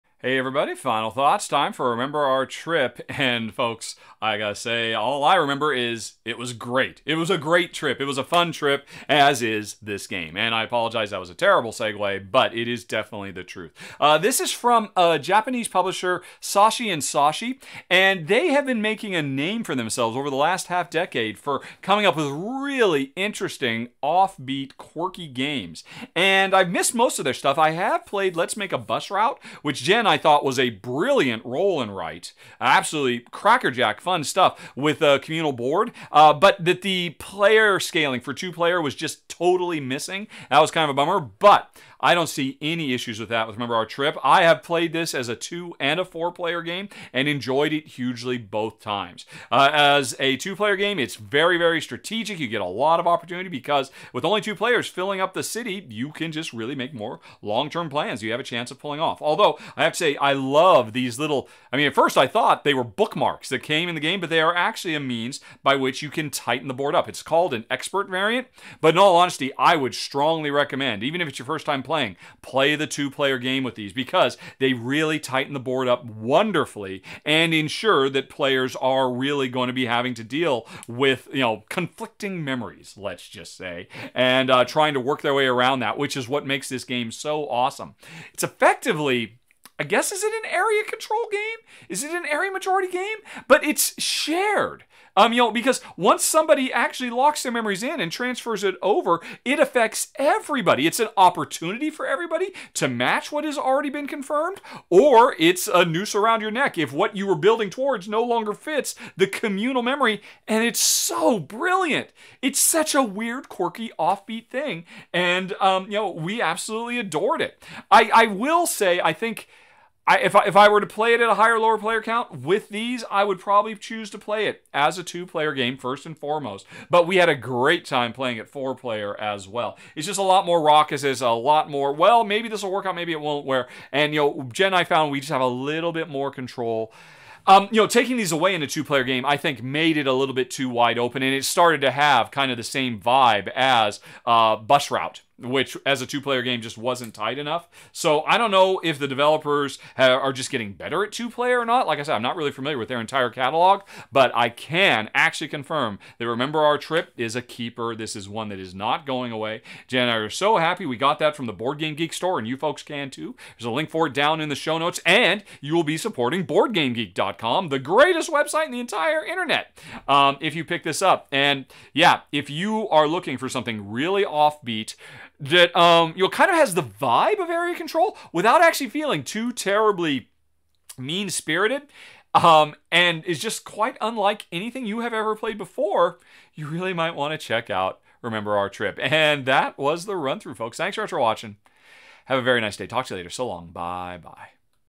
Remember Our Trip | Rahdo's Final Thoughts
A video outlining gameplay for the boardgame Remember Our Trip.